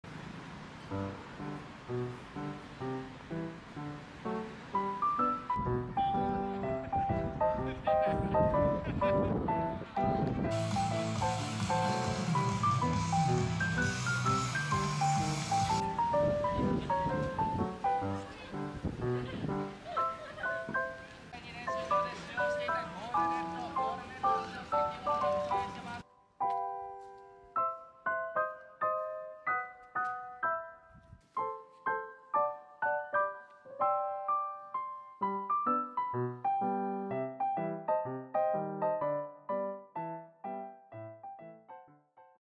3年ぶりに行われたコンサドーレ札幌サポーターズデー/札幌のとある食器屋さん
【サポーターvlog】推しのチームや選手と触れ合う1日🟥⬛️⚽️/札幌にある小さな食器屋さん🍽